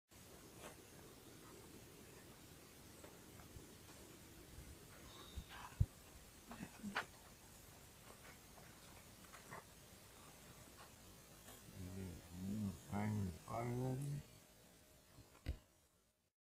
Pervažiuotas žibulis
Automobilio pervažiuotas gluodenas (žibulis) ant miško kelio, netoli Judraičių kaimo. Zapyškio seniūnija, Kauno rajonas, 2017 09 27, 17:23.

Slow Warm (injured by car) on the forest road in Kaunas district, Lithuania.